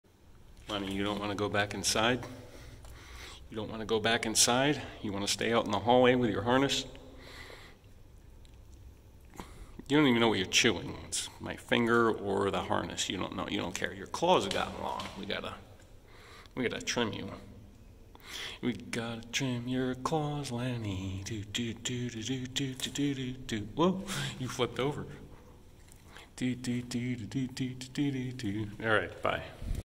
Lenny stays in the hallway just to play with the harness and my hand.
Lenny won't stop playing with my hand and go inside from his hallway excursion.